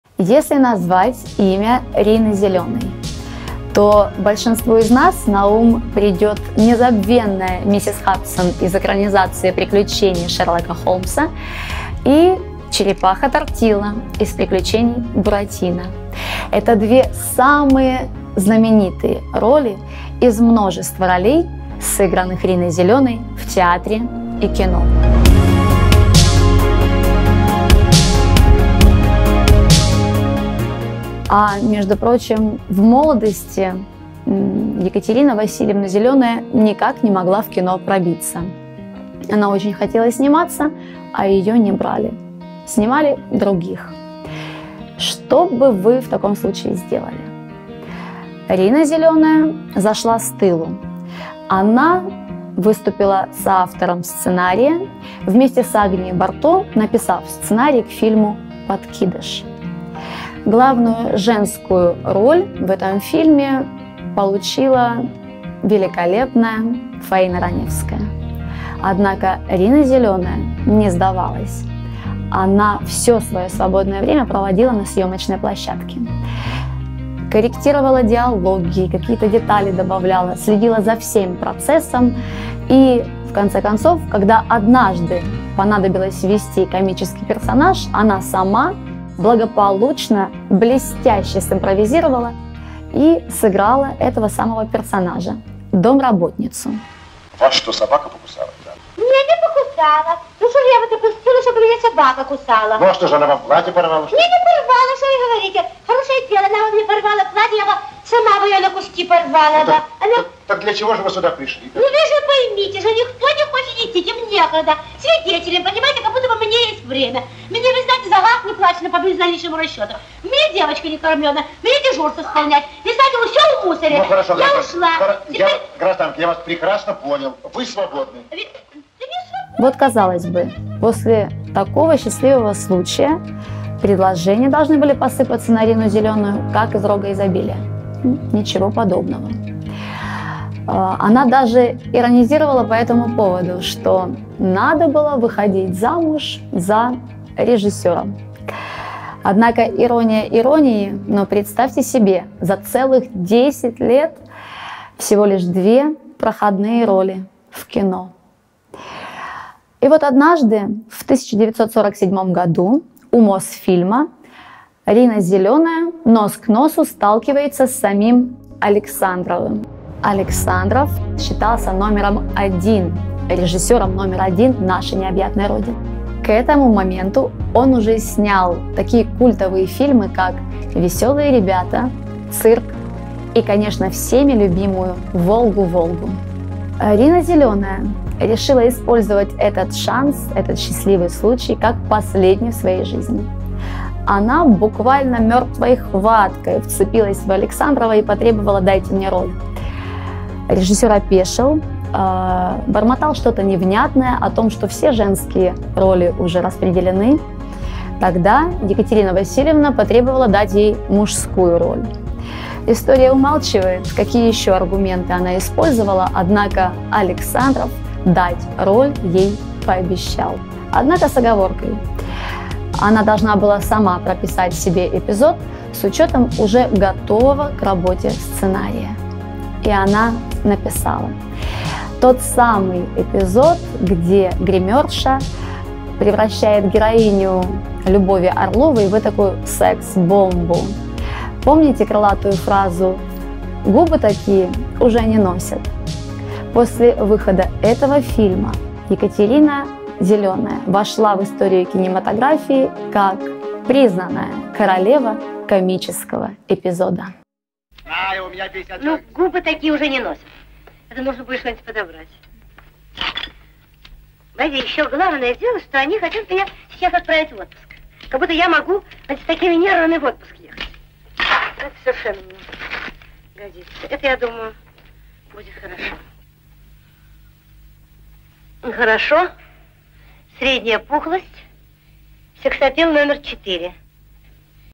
РИНА ЗЕЛЁНАЯ - королева комического эпизода
Сегодня поговорим об истории успеха Рины Зелёной, о случае, который помог ей стать "королевой комического эпизода".